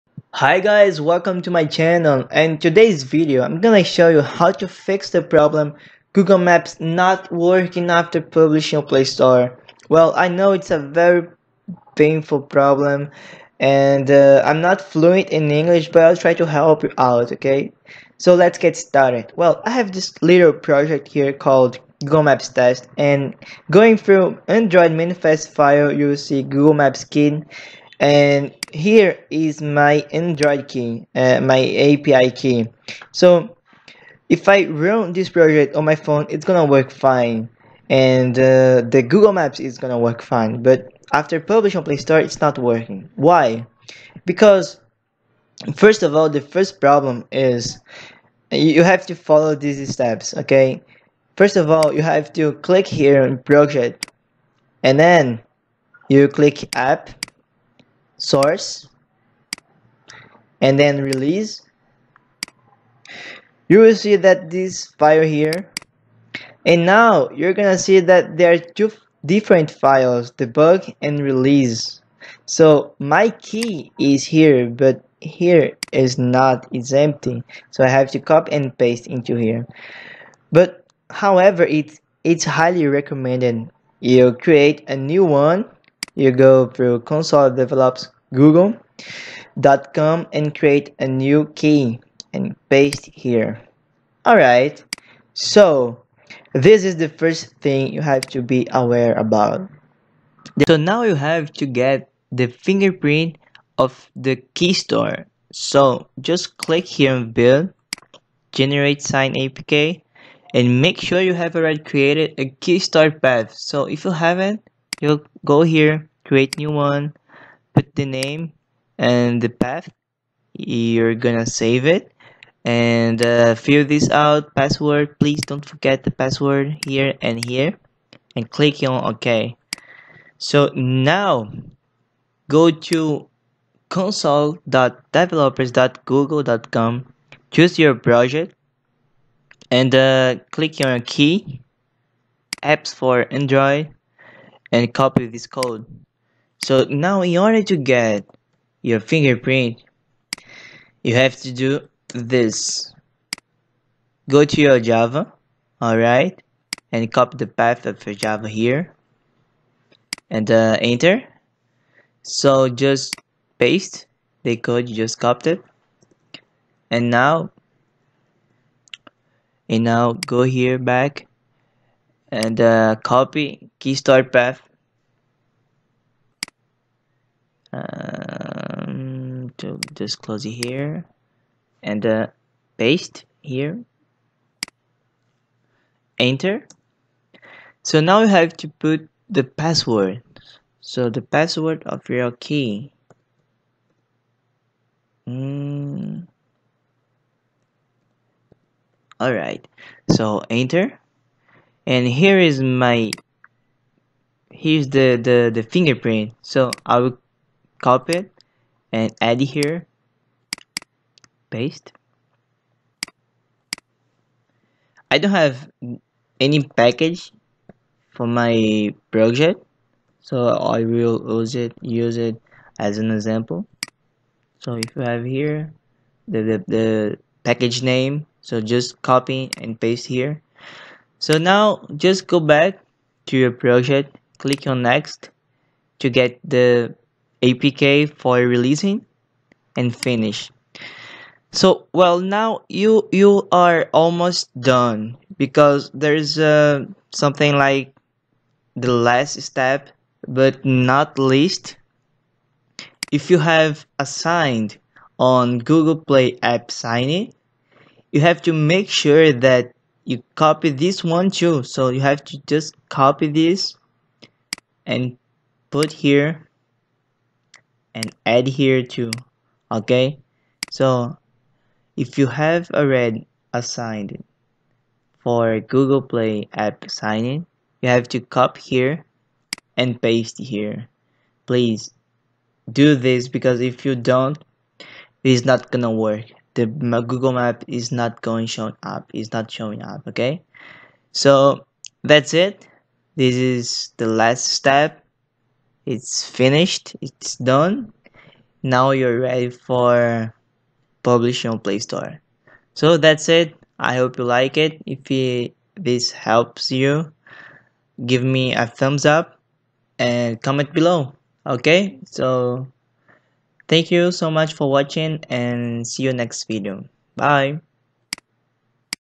Fix - Google maps not showing up after publishing on PlayStore
Hi Guys!

follow this steps and the maps is going to show up after publish on playstore:

1 - make sure you've added your key into release/google_maps_api.xml

2- create a keystore

3 get the SHA1 Fingerprint of keystore you've created

4- copy the sha1 fingerprint and add to your key, by console.developers.google.com

5- build your release app

6 - if you have signed "Google Apps Signing " on playStore, make sure you add the  generated fingerprint to the api key you're using for releasing.

That's it